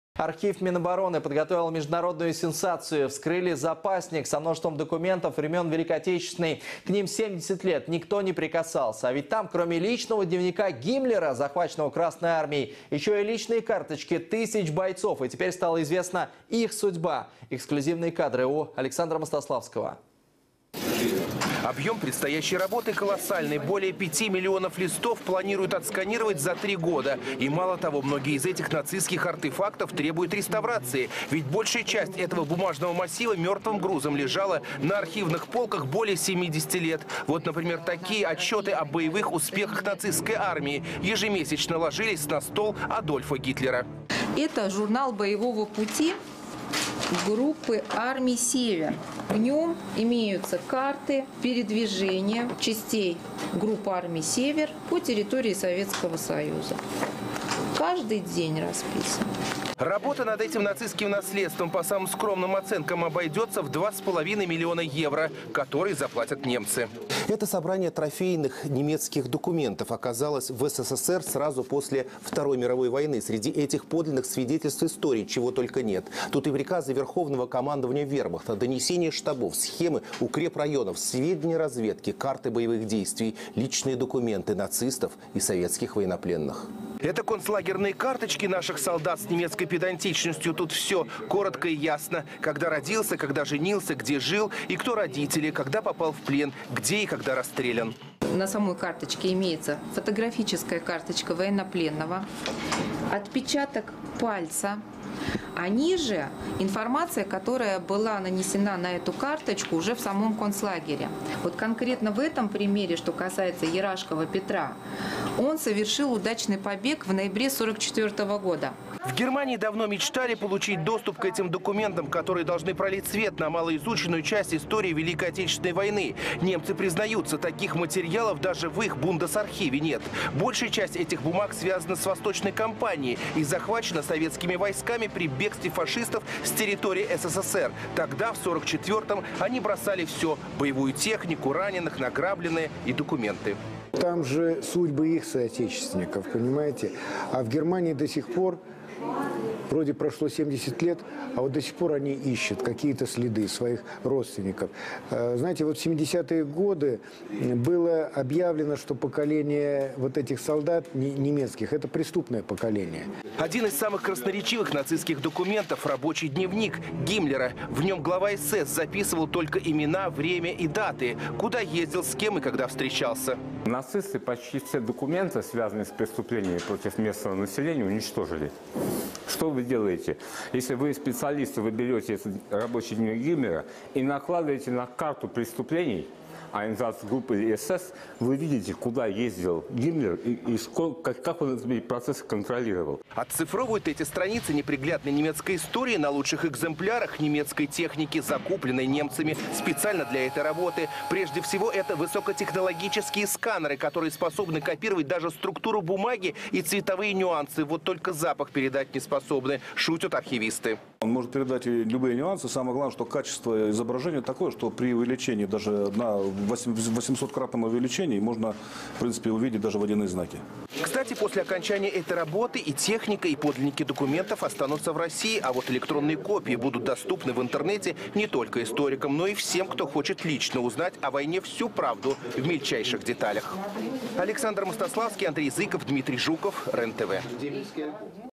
Минобороны раскрыл личный дневник Гимлера, захваченный Красной армией и ещё тысячи документов
А так же не забывайте подписываться на наш канал на YouTube.